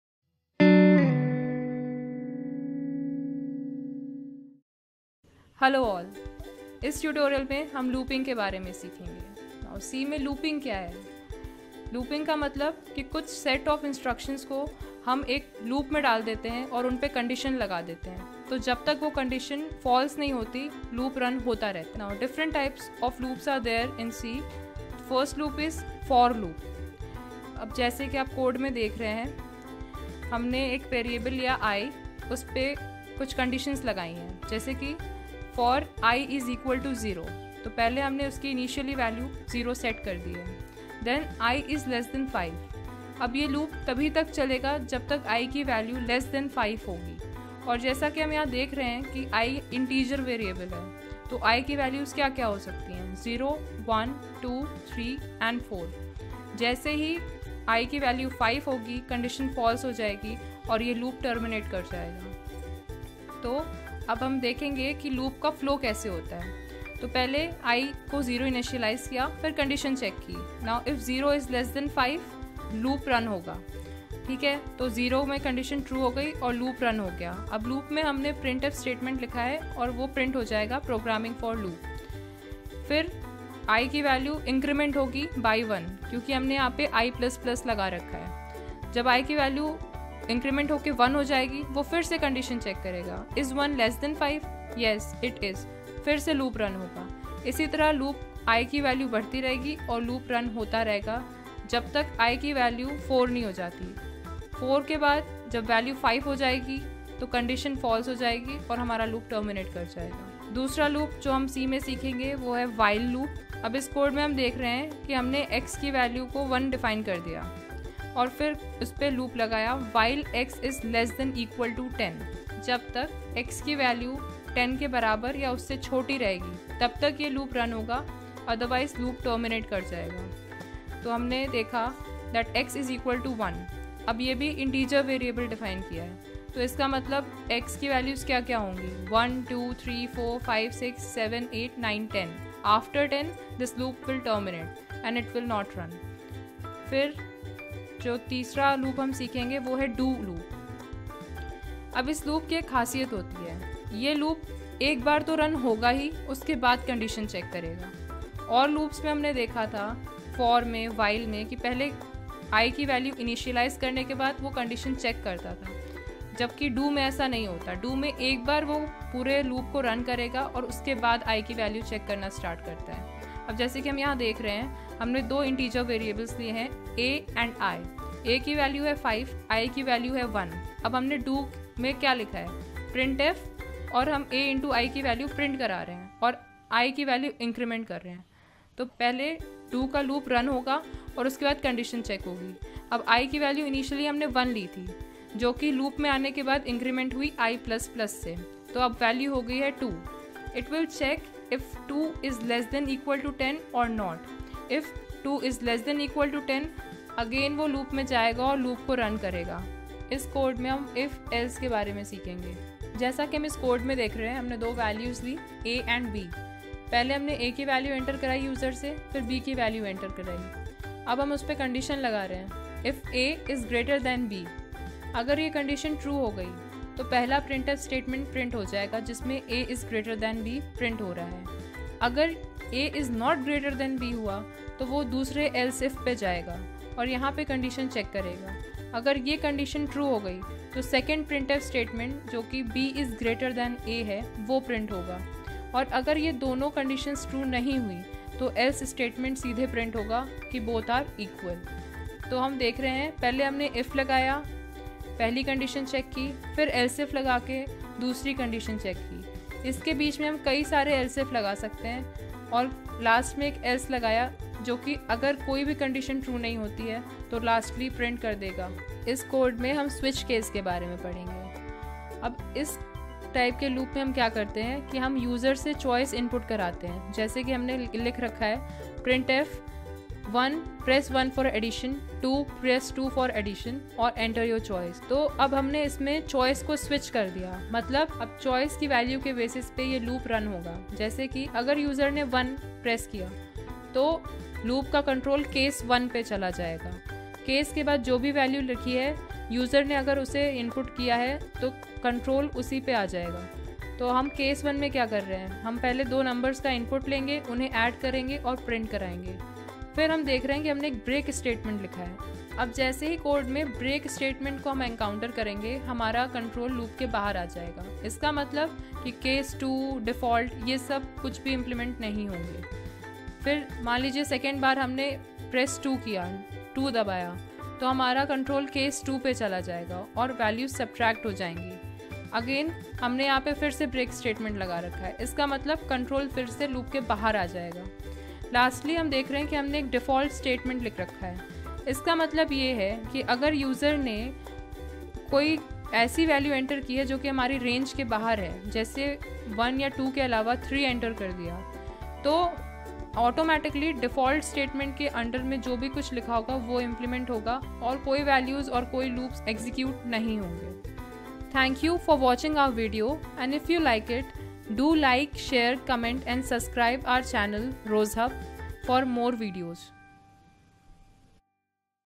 C Made easy | Loops in C | By Pragya and Aditi | S01E05 | Hindi
“‘C’ made easy” is a tutorial program for beginners in programming. Since, ‘C’ is called ‘The Mother of all Programming Languages’ our Interns Aditi and Pragya decided to help students to learn about coding in a simpler way, with this 10 episode series during their one month internship at Rosehub.
HAPPY CODING !